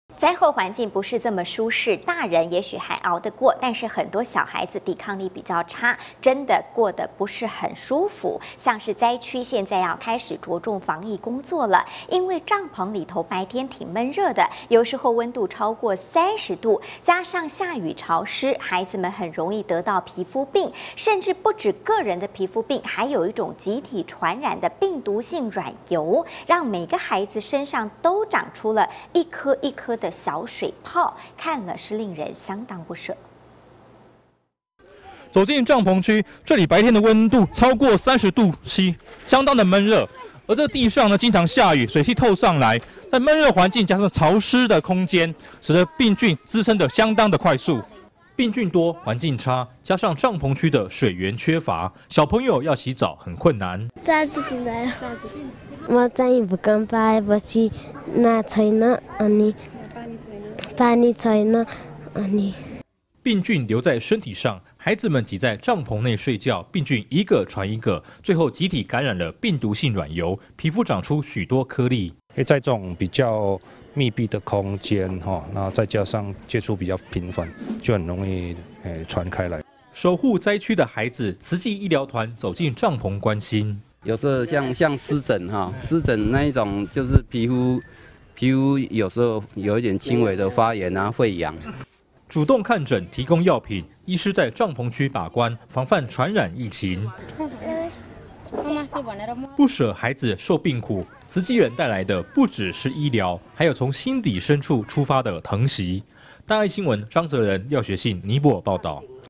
【慈善】潮溼悶熱病茵孳生孩童染皮膚病
潮濕悶熱 ，有些孩子得到皮膚病，甚至集體感染「病毒性軟疣」，看了令人相當不捨，慈濟醫療團醫護人員得知後，相當關心，前往帳棚區診療。

 走進帳棚區，這裡白天的溫度超過三十度C，相當的悶熱，而這地上經常下雨，水氣透上來，在悶熱環境，加上潮濕的空間，使得病菌滋生得相當快速。

 病菌多，環境差，加上帳棚區的水源缺乏，小朋友要洗澡、很困難。

 巴塔普受災兒童：「我四天前有洗澡。」

 巴塔普受災兒童：「地震後，我都沒水洗澡。」

 病菌留在身體上，孩子們擠在帳棚內睡覺，病菌一個傳一個，最後集體感染了「病毒性軟疣」，皮膚長出許多顆粒。

 慈濟賑災醫療團醫師 黃俊卿：「在這種比較密閉的空間， 再加上接觸比較頻繁，就很容易傳染開來。」

 守護災區的孩子，慈濟醫療團、走進帳棚關心。

 慈濟賑災醫療團醫師 葉添浩：「有時候一些細菌性的感染， 像濕疹，那種就是皮膚，會有輕微的發炎 會癢。」

 主動看診，提供藥品，醫師在帳棚區把關、防範傳染疫情。

 不捨孩子受病苦，慈濟人帶來的、不只是醫療，還有從心底深處出發的疼惜。

 張澤人 廖學信 尼泊爾報導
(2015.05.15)